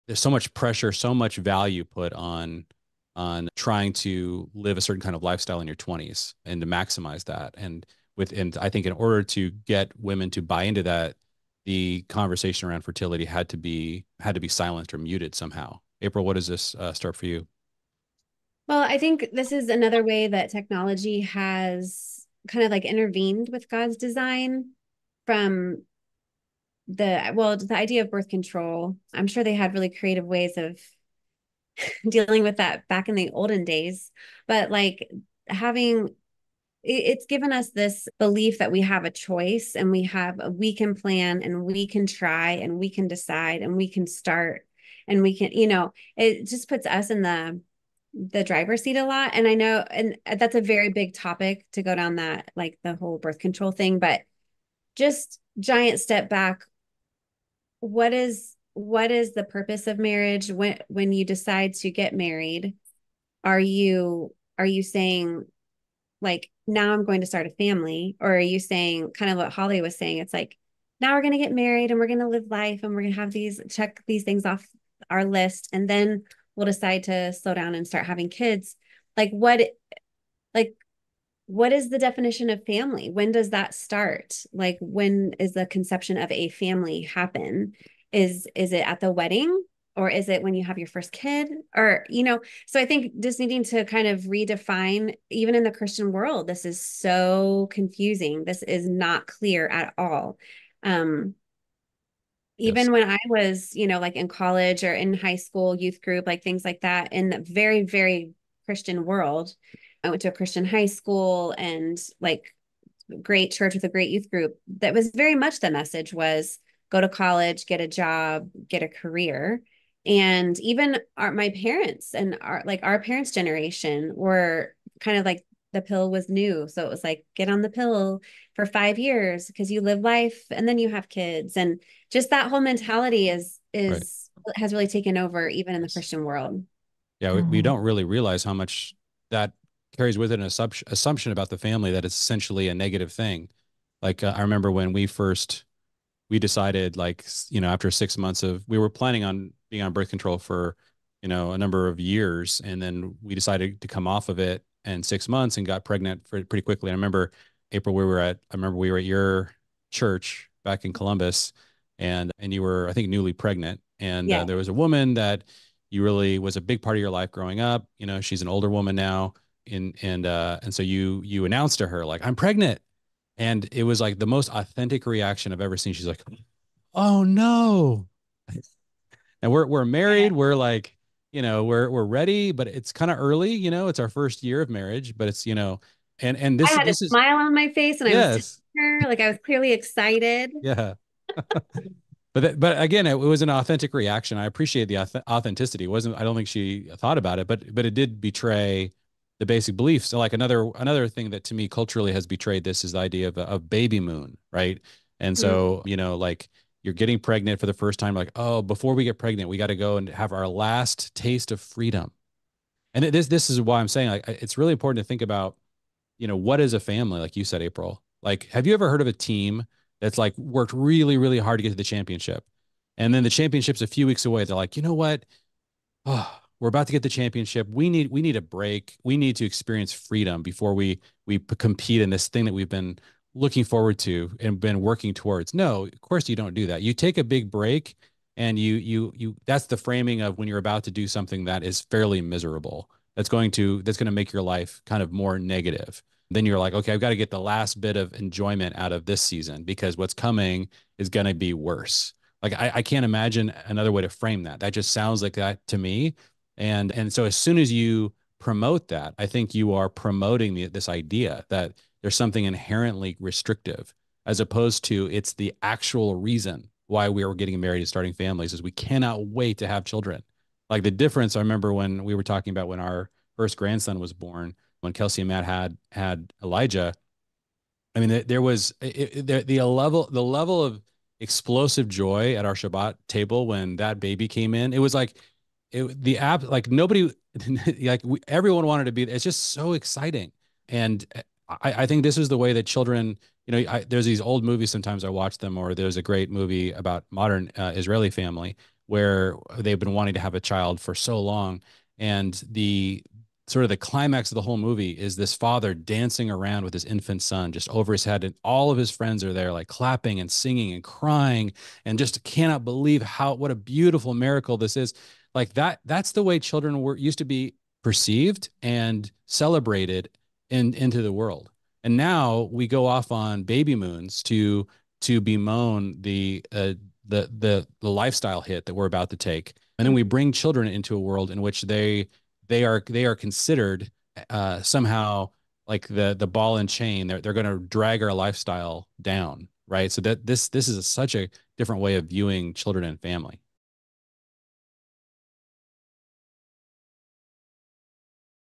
Children Are NOT A Burden...The Right Way To Think About Family [CLIP]
There's so much societal pressure on young women to delay having a family. Everyone, even Christians, are telling young people that it's a burden to have kids, that they should enjoy some freedom when they're young, and that having kids is like a kind of death that they should avoid.

WRONG!

We have to get back to our original design for family and CELEBRATE every child a young mother and father bring into the world.